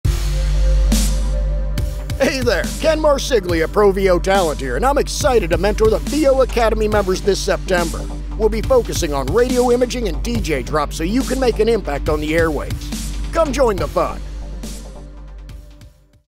Boost Your VO Skills: Radio Imaging & DJ Drops with Ken Marsiglia
🚨 September's VO Academy Mentor Alert! 🚨

This month, we are thrilled to welcome Ken Marsiglia, a seasoned voiceover artist with 7 years of experience and a portfolio boasting over 4000 professional voiceovers. Ken has lent his voice to movie and game trailers, radio promos, commercial TV spots, and much more. He's worked with top-tier clients like Panasonic, O'Reilly Auto Parts, Mattel [Hot Wheels], FunPlus Games, and many others.

September's focus is all about making an impact on the airwaves with radio imaging and DJ drops. With Ken’s expertise in audio production and sound design, you'll be in great hands to elevate your VO game!
Join the fun and add another tool to your VO biz. 🚀